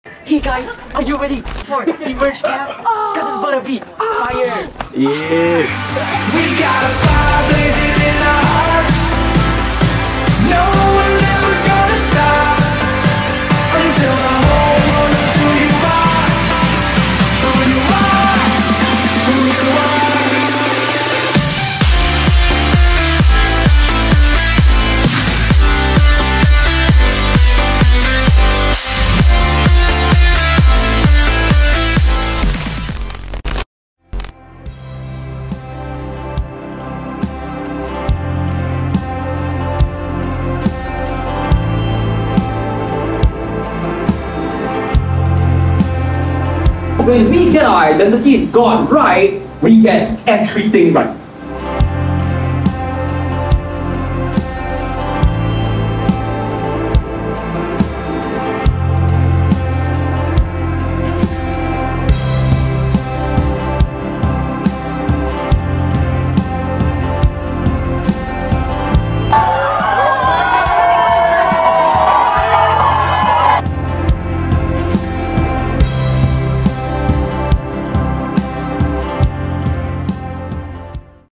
EMERGE Camp 2024: REIGN Recap ✨
Christ reigns in me, I reign in life. 👑

We had an awesome time at EMERGE 2024 last week - with 91 youths,  8 declaration to follow Christ for the rest of their lives, and 3 salvations!